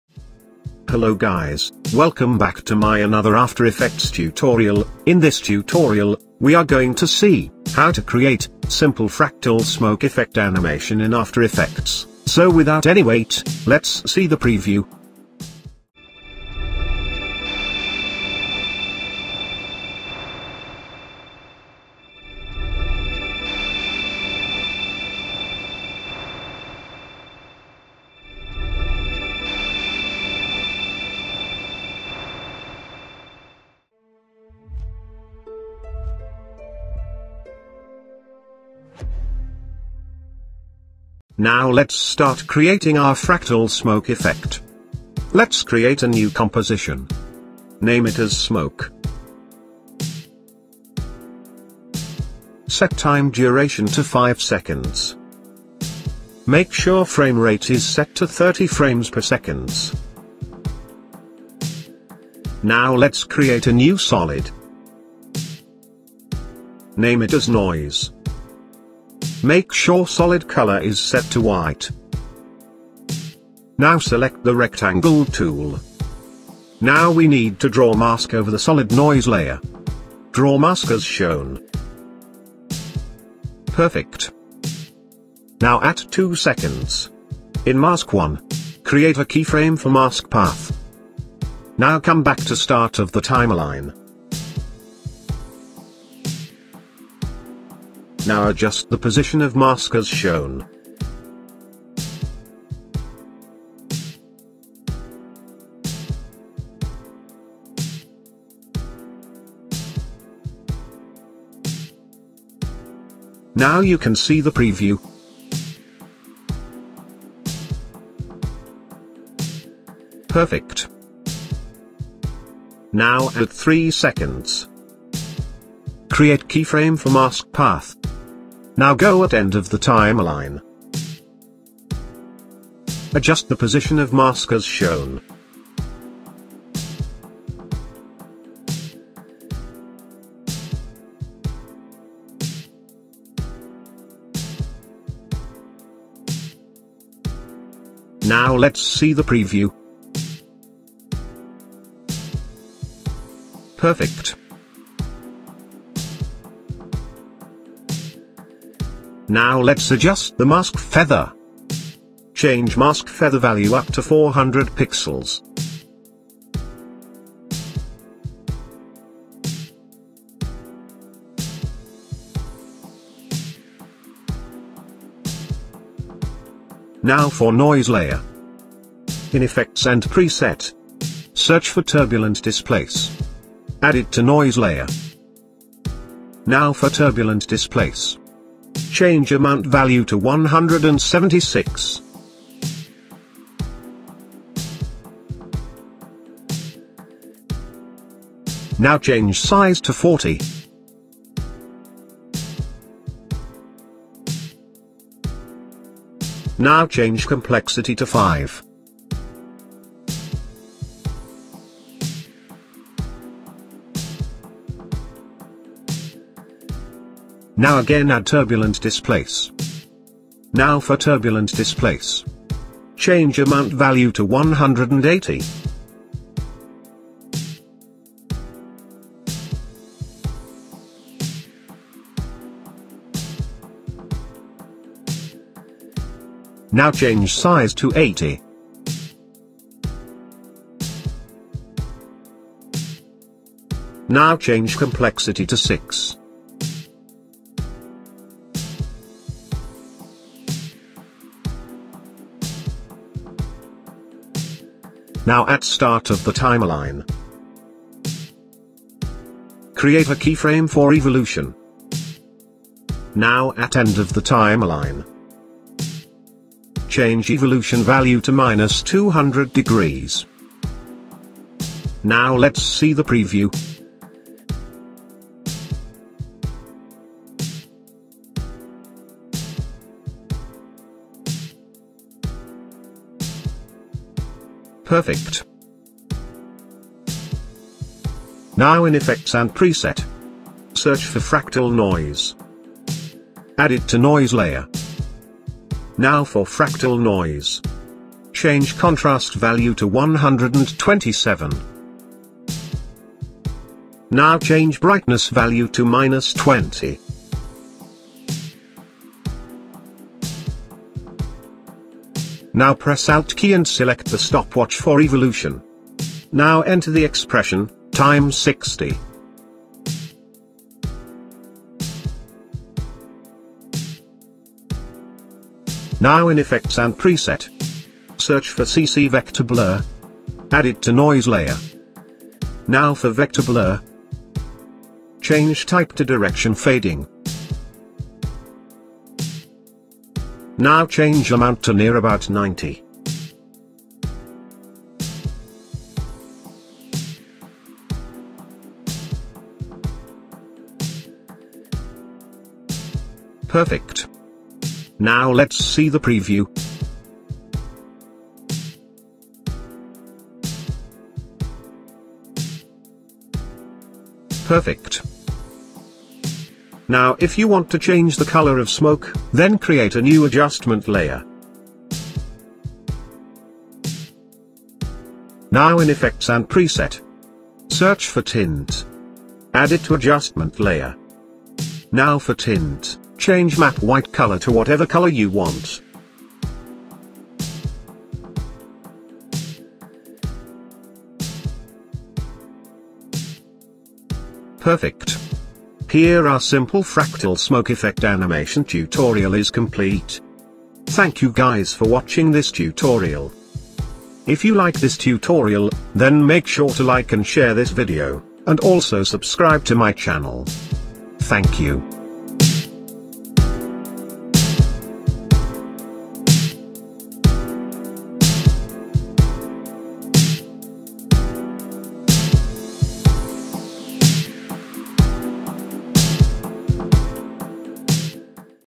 After Effects Tutorial: Fractal Smoke Effect Animation In After Effects
Hello friends, this tutorial is about how to Create a Simple Fractal Smoke Effect Animation In After Effects. It is the Simple Way without the use of any third party plugin